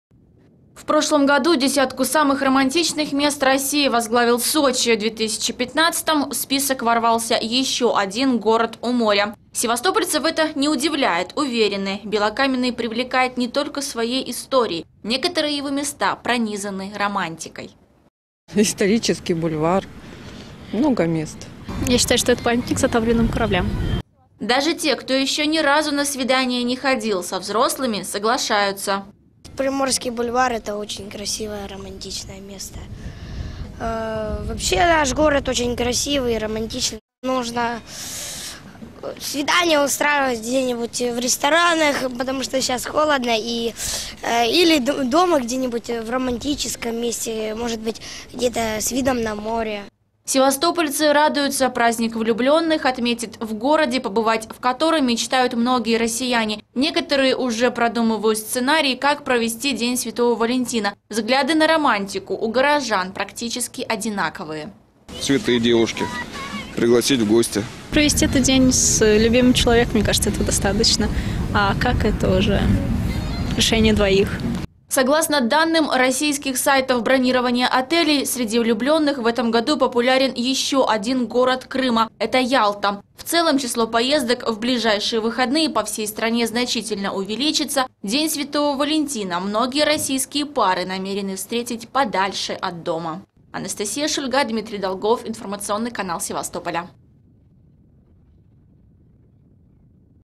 Севастополь попал в десятку городов России
Севастополь попал в десятку городов России, где бы влюбленные хотели отметить приближающийся День Святого Валентина. Рейтинг составил один из популярных сайтов бронирования отелей. Согласно ему, пока что российские влюбленные отдают предпочтение Санкт-Петербургу. За ним следуют Москва, Сочи, Казань, Екатеринбург, Суздаль, Вологда, Кострома, Иркутск и Севастополь.